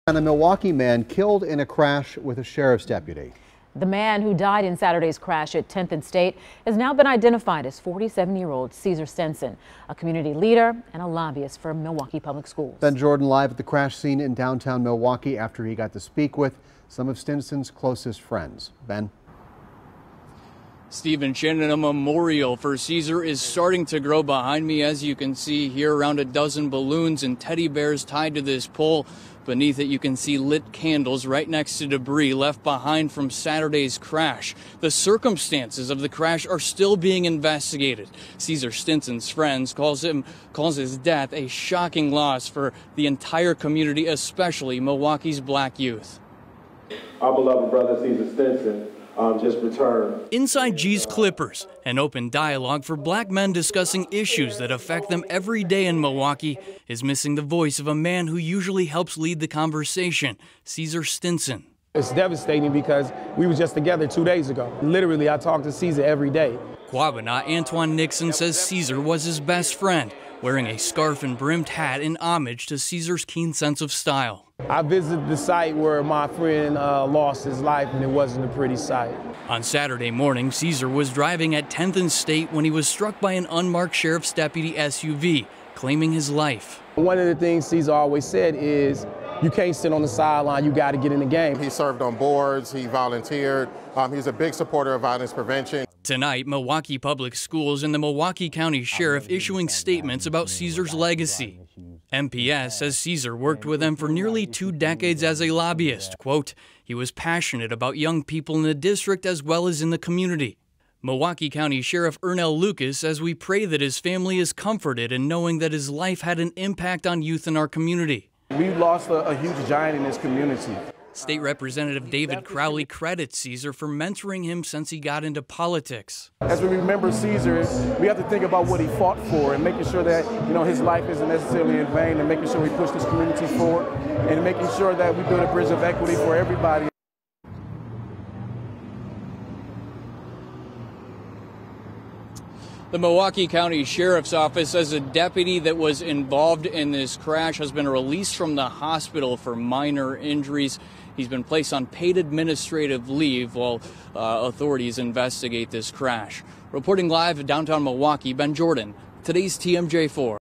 'We've lost a huge giant in this community:' Friends remember man killed in deputy-involved crash
The man who died in a deputy-involved crash Saturday at 10th and State has been identified as 47-year-old Ceasar Stinson. Stinson is well known in Milwaukee for his work as a community leader and lobbyist for Milwaukee Public Schools.